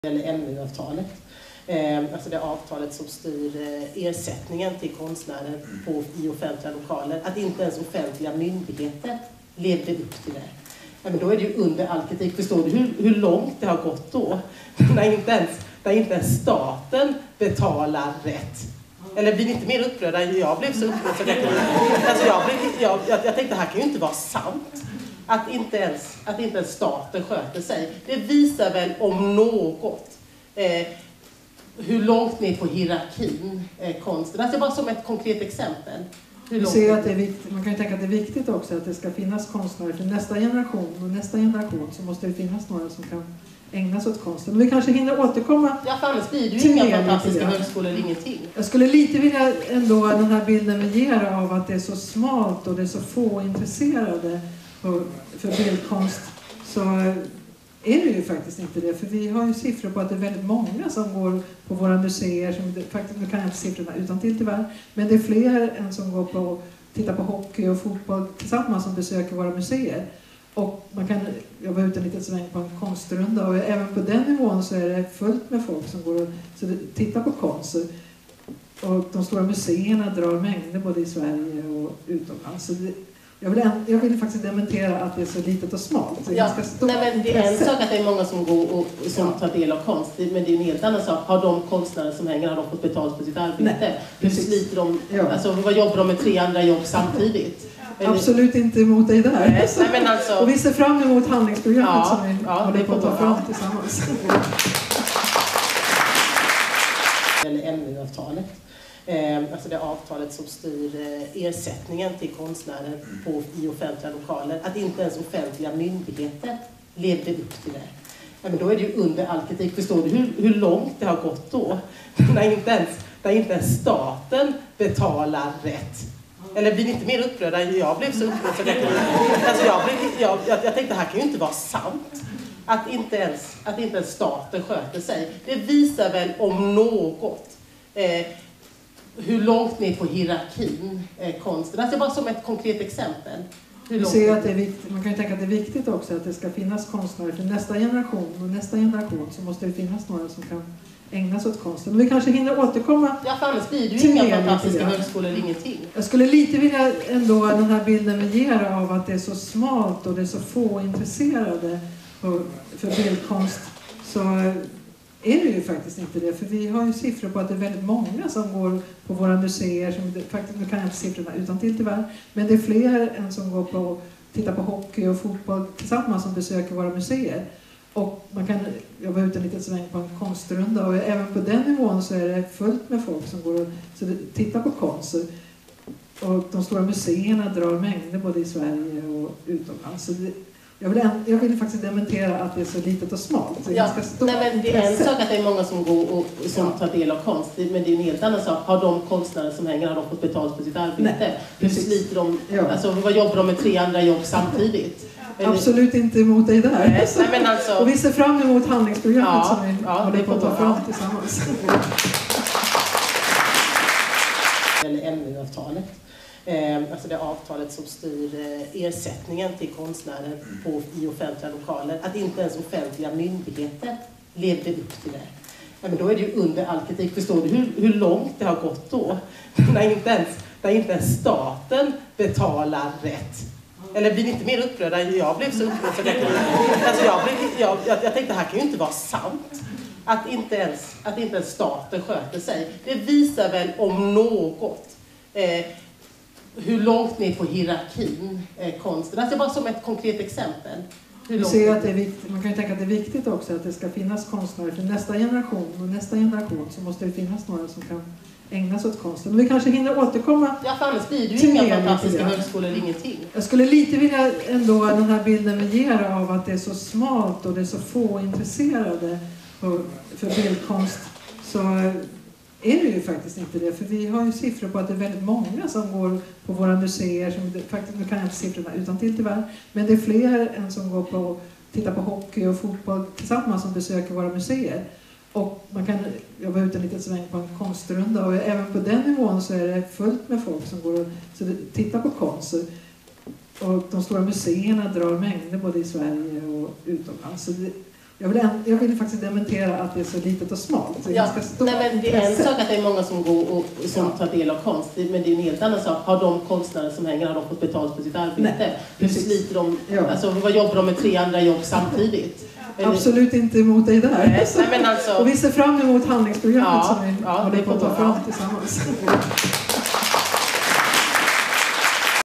WORLD ART DAY - MU-avtalet loop
WORLD ART DAY 2015
Kulturminister Alice Bah Kuhnke börjar spontant prata om MU-avtalet på World Art Day i april 2015:
”Hur långt det gått då? När inte ens staten betalar konstnärerna rätt. Det är under all kritik.”

Samtalsledare: Katarina Jönsson Norling, konstnär och riksordförande för Konstnärernas Riksorganisationer

Fakta MU-avtalet:
År 2009 ingick staten och konstnärsorganisationerna ett ramavtal – MU-avtalet – för att garantera konstnärer anständiga ersättningar vid utställningar. M:et står för Medverkanersättning och inkluderar bland annat den arbetstid en konstnär investerar i en utställning. U:et står för utställningsersättning och är en upphovsrättslig visningsersättning.